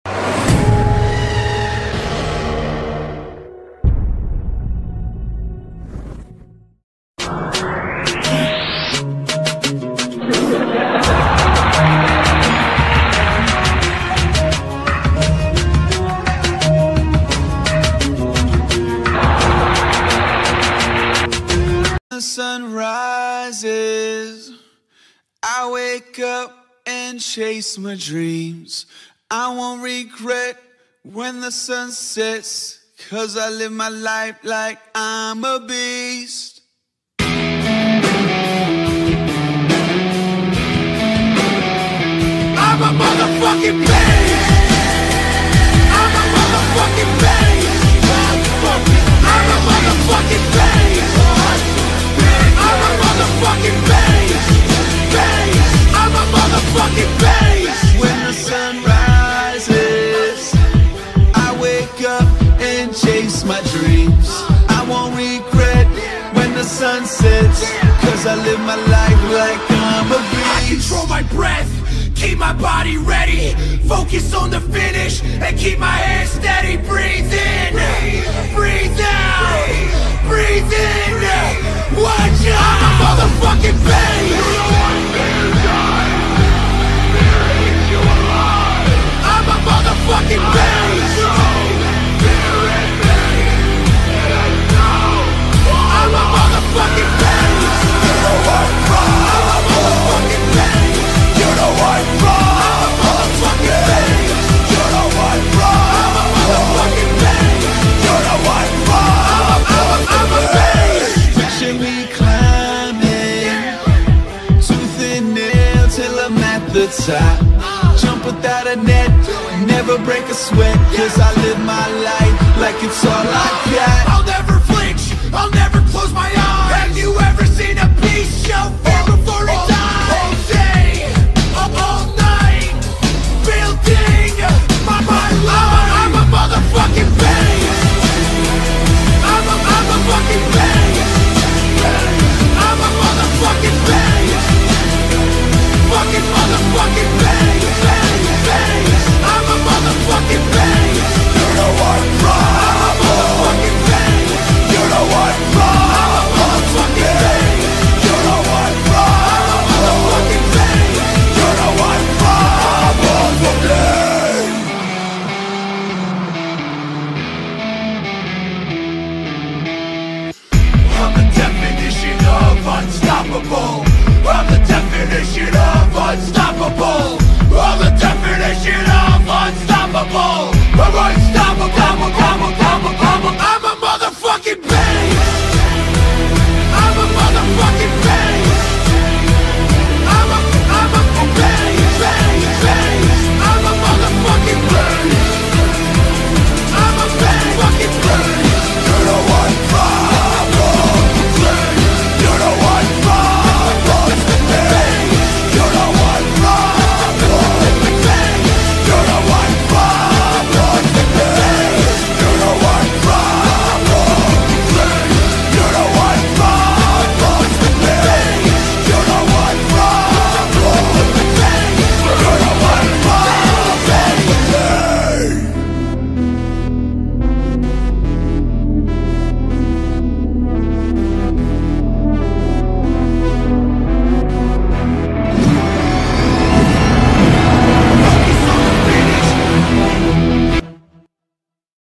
GYM Motivational Video
HELLO ALL !

                      This entire part of the video is made to inspire you in the sense of a gymnasium , in which someone is trying hard to achieve their goal.